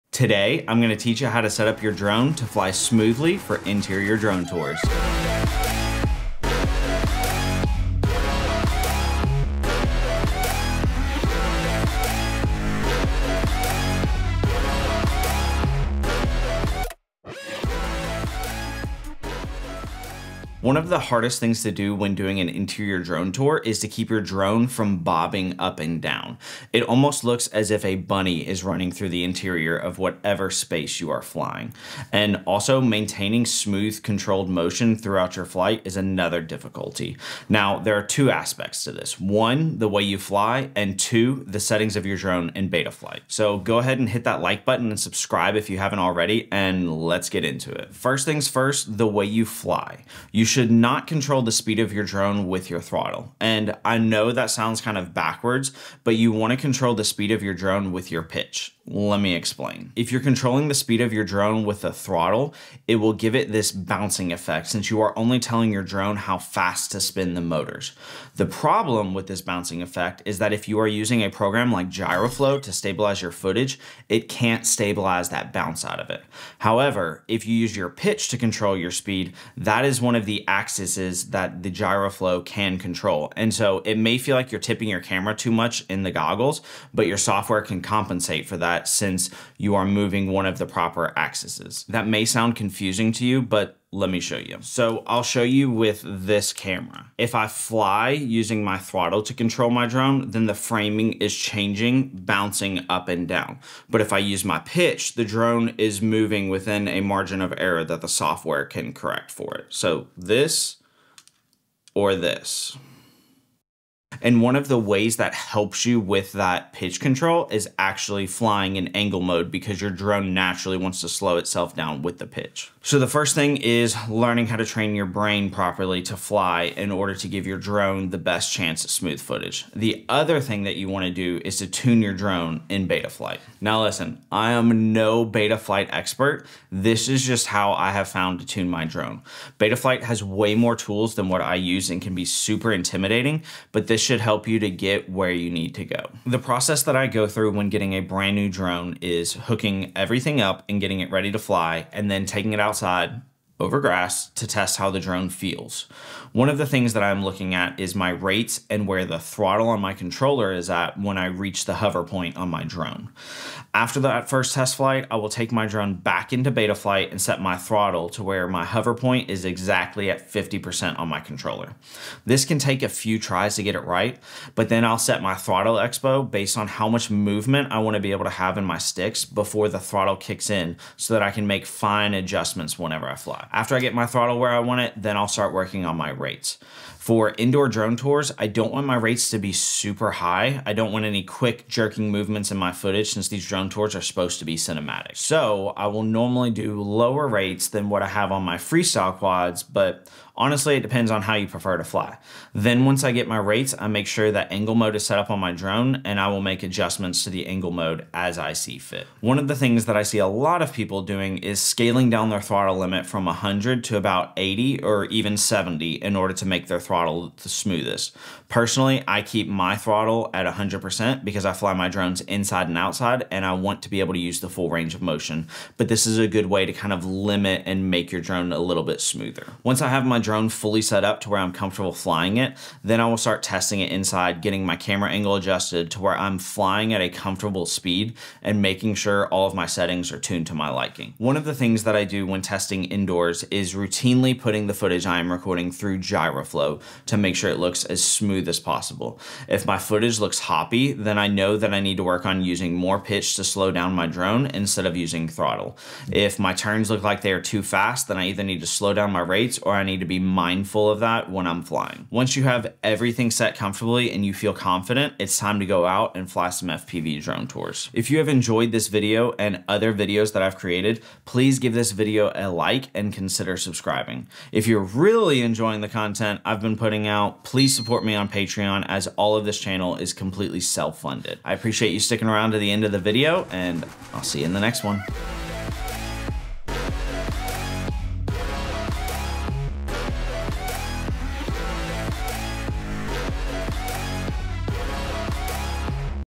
From Bounce to Flow / FPV Drone Tour Betaflight Settings
Hopefully this helps with setting up your drone in betaflight.  The program can be confusing and it takes some trial and error but once you lock it in your fpv drone flying with be smooth as can be!

Gear I use:
Drone: Flywoo Flylens 85
Controller: TBS Tango 2
Goggles: DJI Goggles V2
Antenna: Iflight Crystal HD
Drone Camera: DJI 03 Air Unit
Camera: Sony FX3
Mic: Deity Mic
Bag: Ethix Chesty
Batteries: GNB